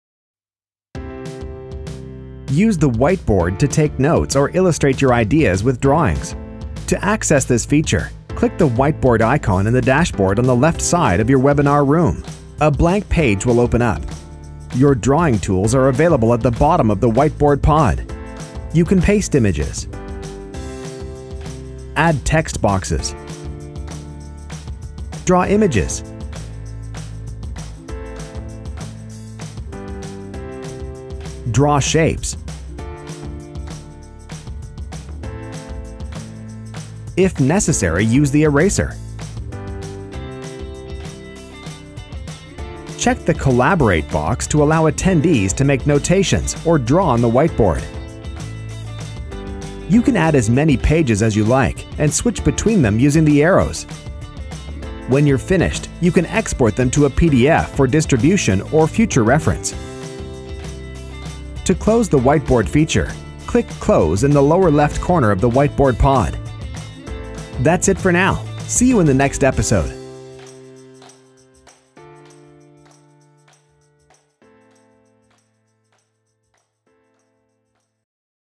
Using webinar whiteboard
Use the Whiteboard to take notes or illustrate your ideas with drawings.
To access this feature, click the whiteboard icon in the dashboard on the left side of your webinar room.
A blank page will open up.
Your drawing tools are available at the bottom of the whiteboard pod.
You can paste images.
Add text boxes.
Draw images.
Draw shapes.
If necessary, use the eraser.
Check the Collaborate box to allow attendees to make notations or draw on the whiteboard.
You can add as many pages as you like and switch between them using the arrows.
When you're finished you can export them to a pdf for distribution of future reference.
To close the whiteboard feature, click Close in the lower left corner of the whiteboard pod.